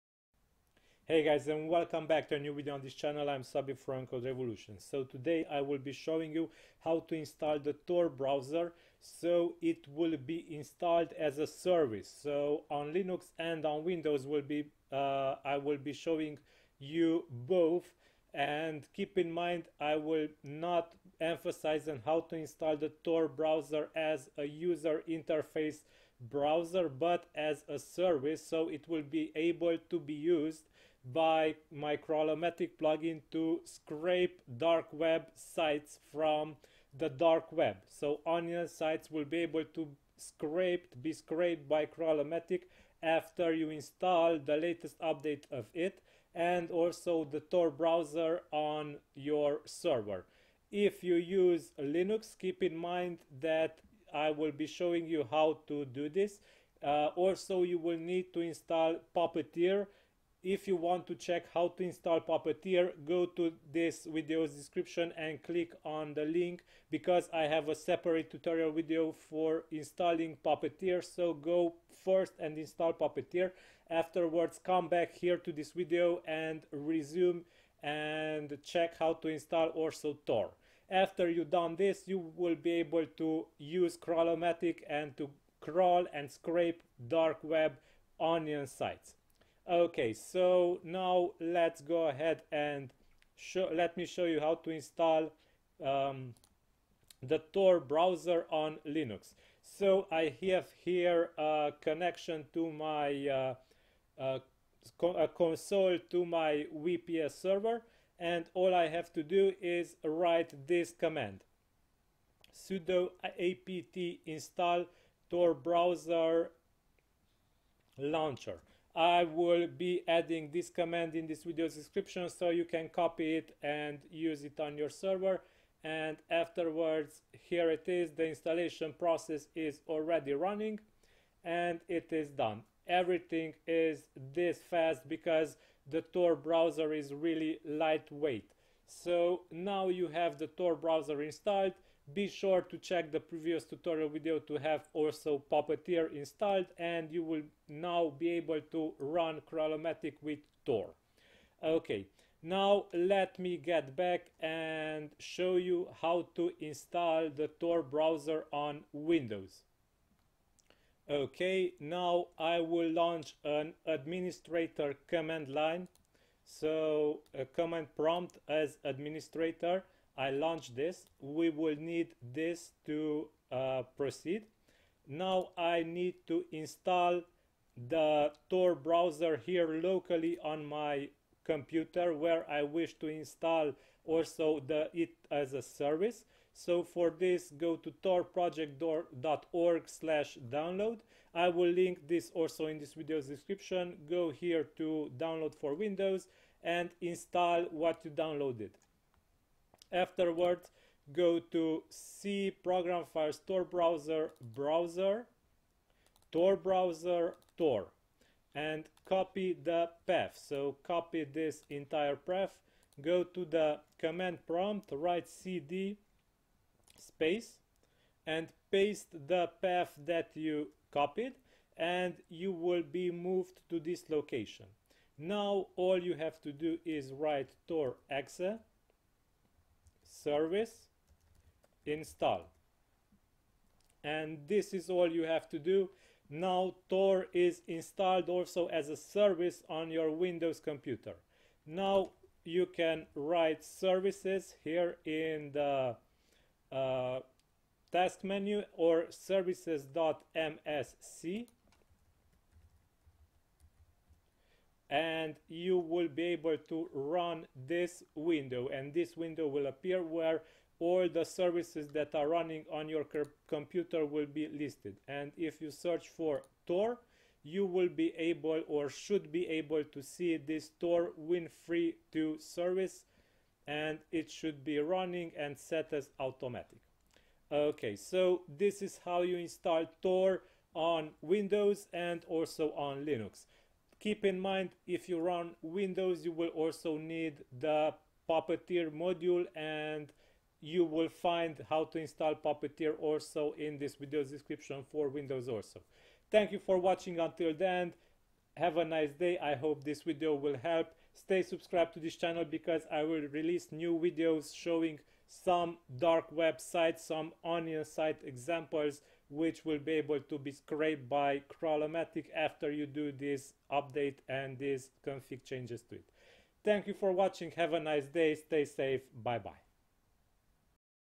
Install the Tor browser on your Linux or Windows Server (to scrape onion sites)
How to install the Tor browser (as a service) on Linux and Windows servers, to scrape onion sites using the Crawlomatic plugin!

Don't forget to install also Puppeteer for this to work!

🤔 ABOUT THIS VIDEO 👇
These are the installation instructions for installing Tor on Windows or Linux without the need to have the Tor Browser Bundle (TBB) running all the time as well.
Running Tor as a service on Windows separately from the Tor Browser Bundle is only useful for users who want to keep Tor running all the time, without having to have the Tor Browser running as well.

Here are the steps to install Tor on your server:
For Linux:
- open a shell console and write:
sudo apt install torbrowser-launcher

Install it
Navigate to the location where the tor.exe is located (usually: C:\Program Files\Tor Browser\Browser\TorBrowser\Tor) and open an Administrator command prompt there
Write the following command:
tor.exe –service install
Now you can open services.msc on your Windows machine and check if the tor service was installed correctly and if it is running (listed as "Tor Win32 Service")

After you done these steps, you are ready to scrape websites from the dark web, using the Crawlomatic plugin!

💭 Add your comments! Ask any questions!

On this YouTube channel, you will see a new video published each day at 7 PM GMT!

Szabi - CodeRevolution.